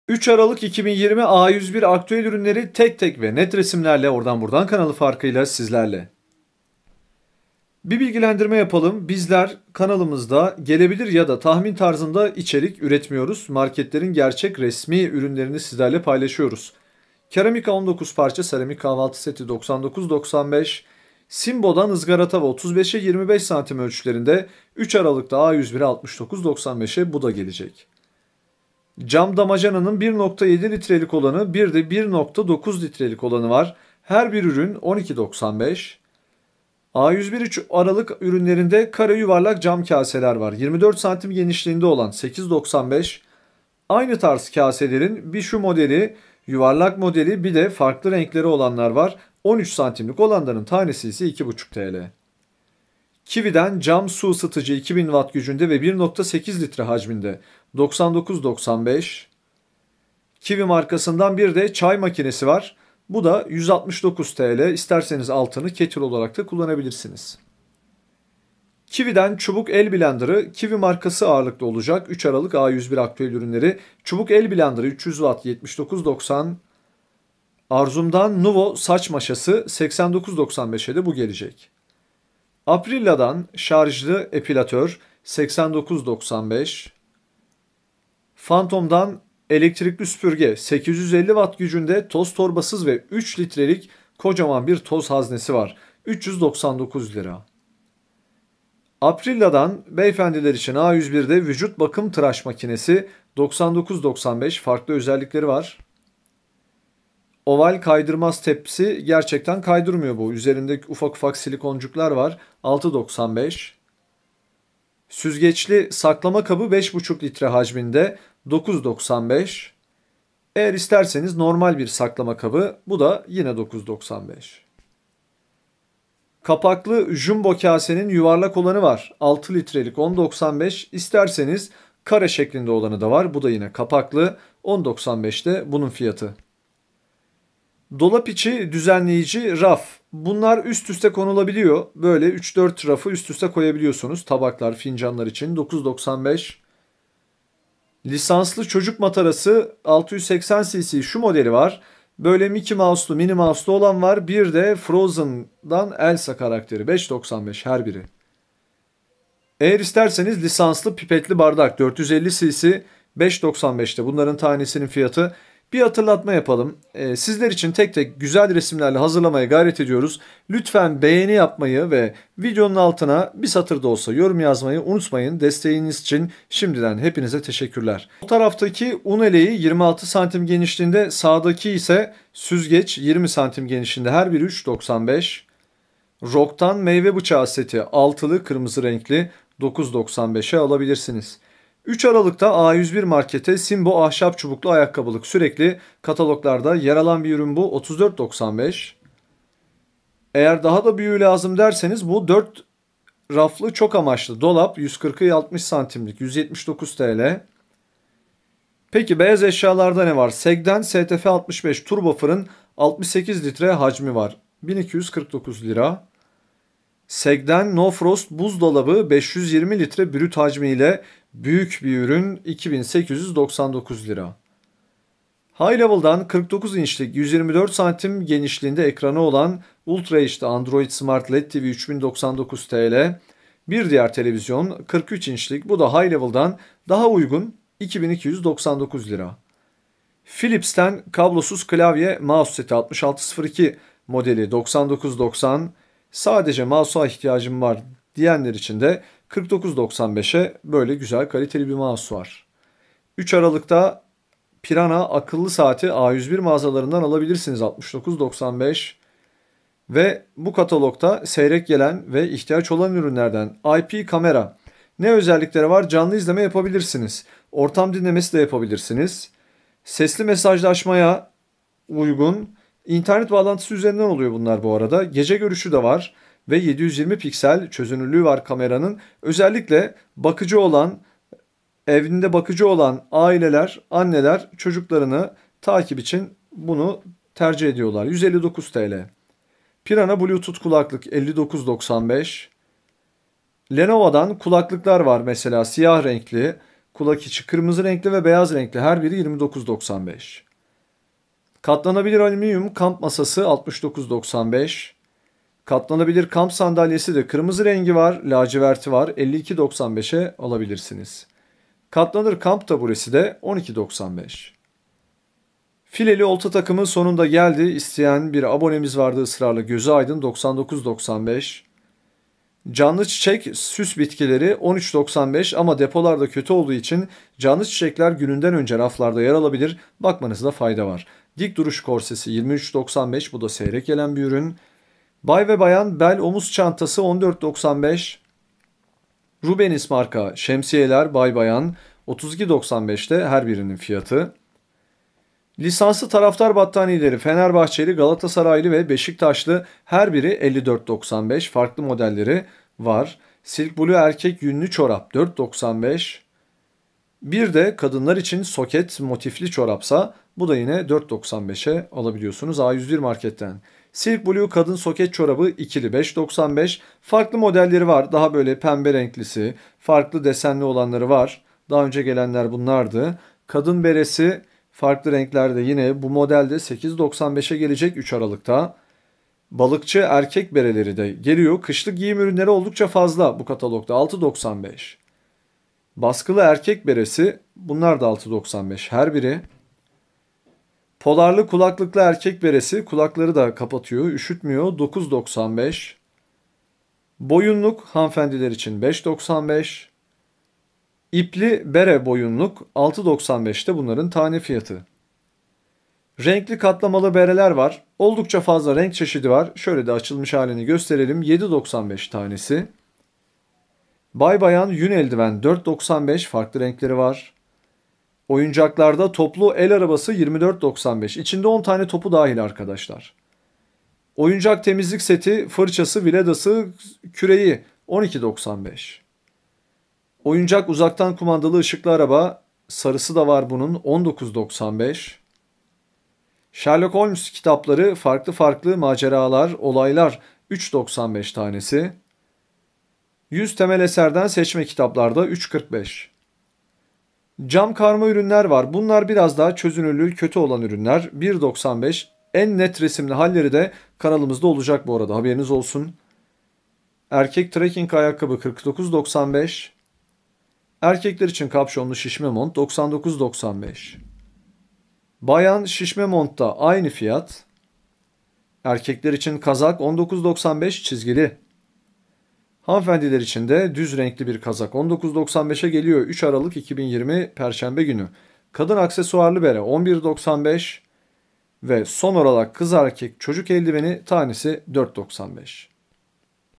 TEK VE NET İZLEYİN | A101 3 ARALIK 2020 | KIŞLIK, MUTFAK ÜRÜNLERİ | A101 İNDİRİMLERİ | {A101 Aktüel}
A101 aktüel ürünleri heyecanla beklenirken kışlık ve mutfak ürünleri ile geldi. Ordan Burdan kanalında 3 Aralık A101  kampanyaları, kataloğu güzelce izleyin diye tek ve net sizler için hazırlandı. Benim de takip ettiğim A101 aktüel aslında bu hafta Perşembe günü de dolu ama A101 10 Aralık 2020 tarihine kadar geçerli katalog fırsatları kaçırılmayacak cinsten. Genel olarak baktığımızda Sinbo Izgara tava, Kiwi markasına ait çaycı, çubuk el blender  ve Aprilla epilasyon aleti göze çarpıyor. Ama Arzum saç maşası ile Piranha ip kamera, bluetooth kulaklık ve Fantom süpüge de fena değil. Kışlık giyim ürünleri içinde bere, kazak ve şişme mont gayet fiyatları güzel.

00:00 | Giriş
00:08 | Ordan Burdan Kanalı Prensibimiz
00:17 | Keramika 19 Parça Seramik Kahvaltı Seti
00:21 | Sinbo Izgara Tava
00:28 | Cam Damacana 1,7 L Ve 1,9 L
00:37 | Kare Yuvarlak Cam Kase 24 cm
00:44 | Kare Yuvarlak Cam Kase 13 cm
00:52 | Kiwi Cam Su Isıtıcı
00:59 | Kiwi Çay Makinesi
01:09 | Kiwi Çubuk El Blenderı 300 Watt Gücünde
01:18 | Arzum AR5066 Nuvo Saç Maşası
01:21 | Aprilla AEP7813 Şarjlı Epilatör
01:27 | Fantom CC3500 Elektrikli Süpürge
01:38 | Aprilla ATR7225 Vücut Bakım Traş Makinesi
01:46 | Oval Kaydırmaz Tepsi
01:54 | Süzgeçli Saklama Kabı
02:00 | Saklama Kabı
02:05 | Kapaklı Jumbo Kase 6 L
02:18 | Dolap İçi Düzenleyici Raf
02:28 | Lisanslı Matara 680 CC
02:40 | Lisanslı Pipetli Bardak 450cc
02:45 | Süzgeç Ve Un Eleği 20 cm 26 cm
03:06 | Meyve Bıçağı Seti 6'lı
03:12 | Sinbo Ahşap Çubuklu Ayakkabılık
03:20 | 4 Raflı Çok Amaçlı Dolap 140 X 60 cm
03:28 | SEG  STF65 Turbo Fırın
03:37 | SEG SNF5200 NoFrost Buzdolabı
03:44 | Hi Level 49 İnç Ultra HD Android Smart LED TV
03:54 | Hi Level 43 İnç Full HD Smart LED TV
04:00 | Philips 6602 Kablosuz Klavye Mouse
04:05 | Philips SPK7344 Kablosuz Mouse
04:13 | Piranha Akıllı Saat
04:20 | Piranha IP Kamera
04:50 | Piranha Bluetooth Kulaklık
04:53 | Lenovo HF130 Kulaklık
05:01 | Katlanabilir Alüminyum Kamp Masası 72 X 55 cm
05:06 | Katlanabilir Kamp Sandalyesi
05:12 | Kamp Taburesi
05:15 | Fileli Olta Takımı
05:22 | Salon Süs Bitkileri Canlı Çiçek
05:29 | Dik Duruş Korsesi
05:35 | Bay Bayan Bel Ve Omuz Çantası
05:38 | Rubenis Bay-bayan Şemsiye
05:45 | Lisanslı Taraftar Battaniyesi
05:52 | Silk Blue Erkek Yünlü Çorap
05:56 | Silk Blue Kadın Soket Motifli Çorap
06:02 | Silk Blue Kadın Soket Çorap 2'li
06:13 | Kadın Bere
06:19 | Balıkçı Erkek Beresi
06:26 | Baskılı Erkek Beresi
06:30 | Polarlı Kulaklıklı Erkek Bere
06:37 | Boyunluk
06:40 | İpli Bere Boyunluk
06:46 | Renkli Katlamalı Bere
06:54 | Bay Bayan Yün Eldiven
06:57 | Oyuncak Toplu El Arabası
07:03 | Oyuncak Temizlik Seti
07:10 | Oyuncak Uzaktan Kumandalı Işıklı Araba
07:17 | Sherlock Holmes Kitapları
07:22 | 100 Temel Eserden Seçme Kitaplar
07:26 | Cam Karma Ürünler
07:37 | Erkek Trekking Ayakkabı
07:41 | Erkek Kapşonlu Şişme Mont
07:45 | Bayan Şişme Mont
07:49 | Erkek Kazak
07:53 | Kadın Kazak
07:58 | Kadın Aksesuarlı Bere
08:03 | Kız Erkek Çocuk Eldiveni

İnstagram hesabımız için takip edebilirsiniz.